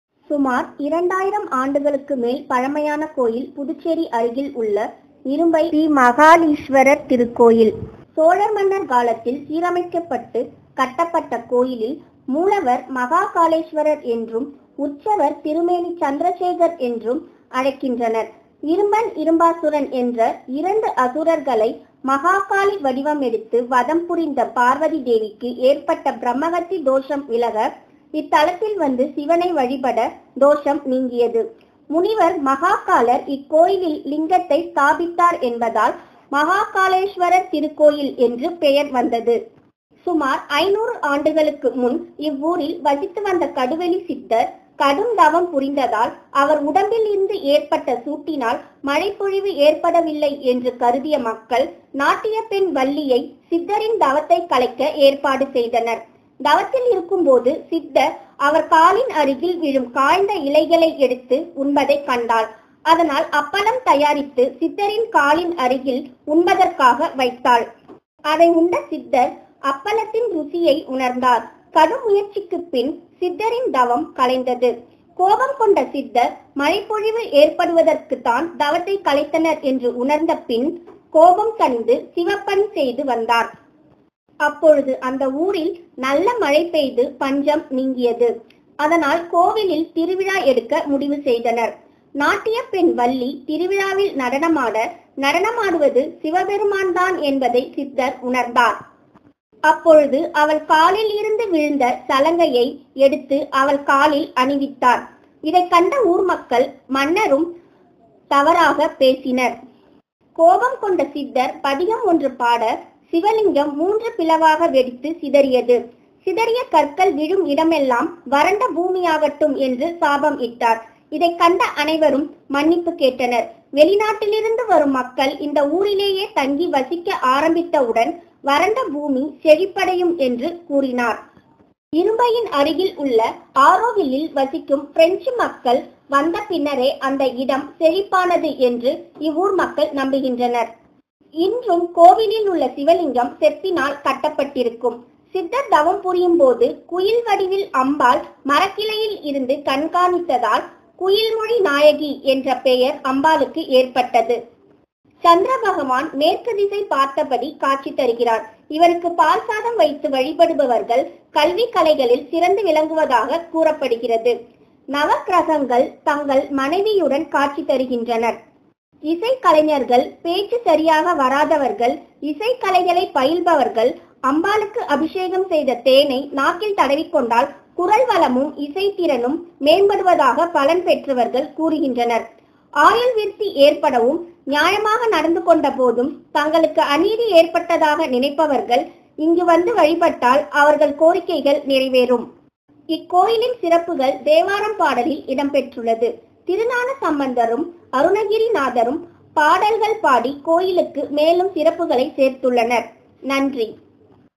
கடுவெளி சித்தர் :இரும்பை ,ஸ்ரீ மதுசுந்தரநாயகி உடனுறை ஸ்ரீ மஹாலீஸ்வரர் திருக்கோயில்
Festivals
Maasi Magam and Shivrathri in Tamil month of Maasi (February-March), Thirukarthikai in Karthikai (November-December) and Panguni Uthiram in Panguni (March-April) are the major festivals celebrated in this temple. Also, all Pradosham are celebrated in a grand manner.
 Timings
 07.00 AM to 12.00 Noon and 04.00 PM to 08.00 PM.
 Address
Shri Mahakaleswarar Temple,
Irumbai Mahalam,
Vanur Taluk, Villupuram District,
Tamil Nadu - 605 111
Tele: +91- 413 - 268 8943, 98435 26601.
Irumbai is about 10 kms from Puducherry and 2 kms away from Morattandi on Puducherry to Tindivanam route. After Morattandi, there is a small road on the right side leading to Irumbai village where the temple is located. Also, the temple is in the vicinity of Auroville International Township.Two more Padal Petra Sthalam, Kiliyanur and Arisili are located very near to this place.
General Information
Moolavar Sri Mahakaleswarar
Ambal --Sri Kuyilmozhi Ammai / Sri Madhu Sundara Nayaki
Theertham (Holy water)
Makalar Theertham
Sthala Vriksham (Sacred Tree)
Punnai (Alexandrian Laurel Tree)
Pathigam (Hymn) rendered by
Saint Thirugnanasambanthar
This is one of the 276 Devara Paadal Petra Shiva Sthalams and 32nd Shiva Sthalam in Thondai Nadu.
Lord Shiva in this temple is a Swayambumurthi (self-manifested).
This is one of the three places where the Shiva Lingam is said to have been installed and worshipped by Mahakala Rishi.
The Vimanam - the tower above the sanctum sanctorum is an excellent example of the Ekadala style of architecture.